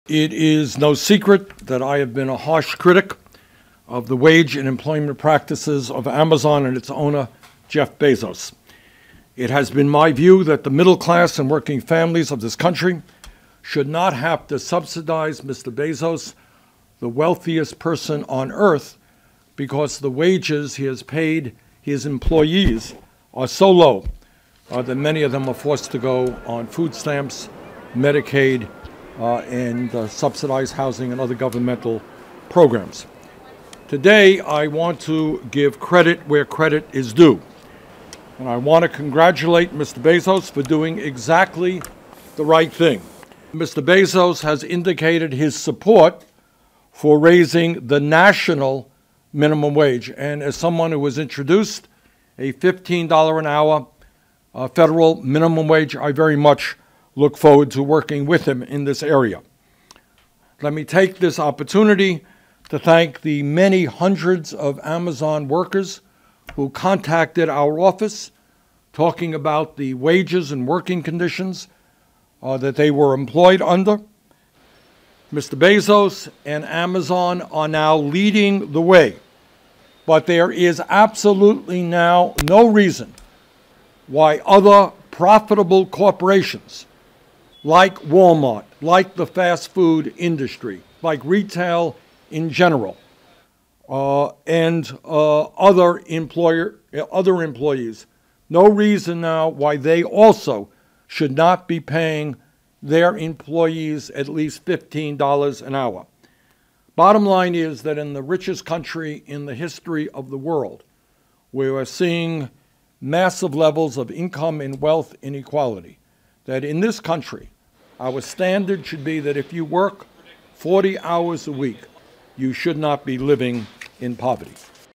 Surprise praise for Amazon from Bernie Sanders
(2 Oct 2018) Long a critic of Amazon.com over its pay practices, Sen. Bernie Sanders is lauding the company for raises that will reach hundreds of thousands of workers. (Oct. 2)